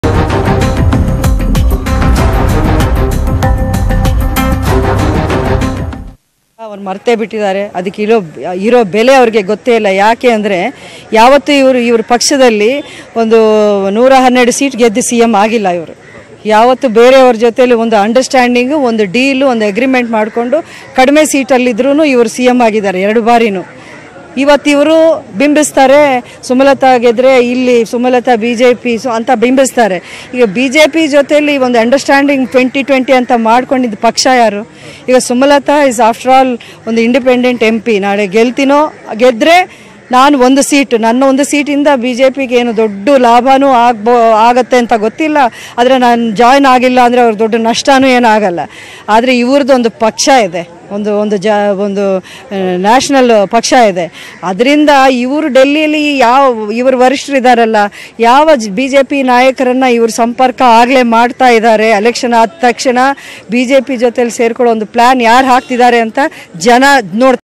112 ಸೀಟು ಗೆದ್ದು ಕುಮಾರಸ್ವಾಮಿ ಸಿಎಂ ಆದವರಲ್ಲ | Kumaraswamy Is Deal CM Says Sumalatha
sumalathaambareesh sumalatha cmkumaraswamy | 112 ಸೀಟು ಗೆದ್ದು ಕುಮಾರಸ್ವಾಮಿ ಸಿಎಂ ಆದವರಲ್ಲ. ಎರಡು ಸಲ ಸಿಎಂ ಆದಾಗಲೂ ಅವರು ಡೀಲ್​ ಮಾಡಿ, ಅಗ್ರಿಮೆಂಟ್​ ಮೇಲೆ ಮುಖ್ಯಮಂತ್ರಿಯಾದವರು. ಈಗ ನನ್ನ ಮೇಲೆ ವೃಥಾ ಆರೋಪ ಮಾಡಿ ದಿಕ್ಕು ತಪ್ಪಿಸುತ್ತಿದ್ದಾರೆ. ನನ್ನ ಬೆಂಬಲ ಸಿಗದಿದ್ರೂ ಬಿಜೆಪಿಗೆ ನಷ್ಟವಿಲ್ಲ. ಆದರೆ, ಬಿಜೆಪಿ ವಿಚಾರದಲ್ಲಿ ಇವರೇನೂ ಸಾಚಾನಾ ಎಂದು ಸಿಎಂ ಕುಮಾರಸ್ವಾಮಿ ವಿರುದ್ಧ ಸುಮಲತಾ ವಾಗ್ದಾಳಿ ನಡೆಸಿದ್ದಾರೆ.